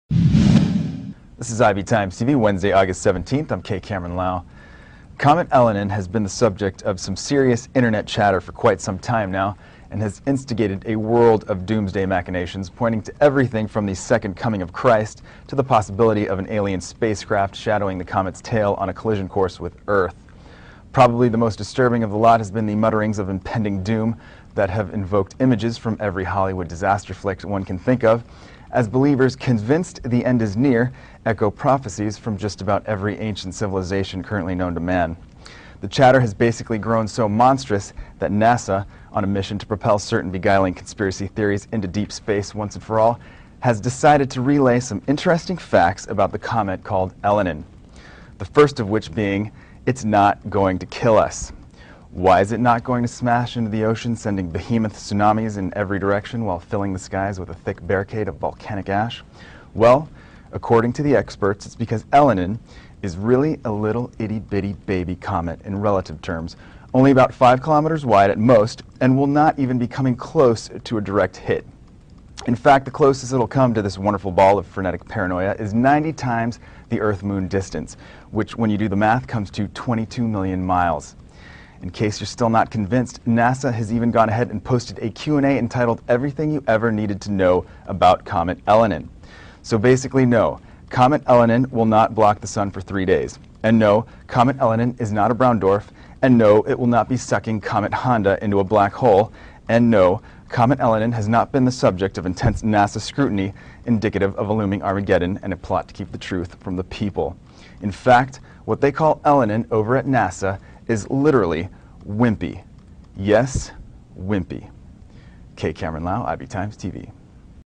NASA Calls Comet Elenin "Wimpy"
"Often, comets are portrayed as harbingers of gloom and doom in movies and on television, but most pose no threat to Earth," NASA officials said. "Comet Elenin, the latest comet to visit our inner solar system, is no exception."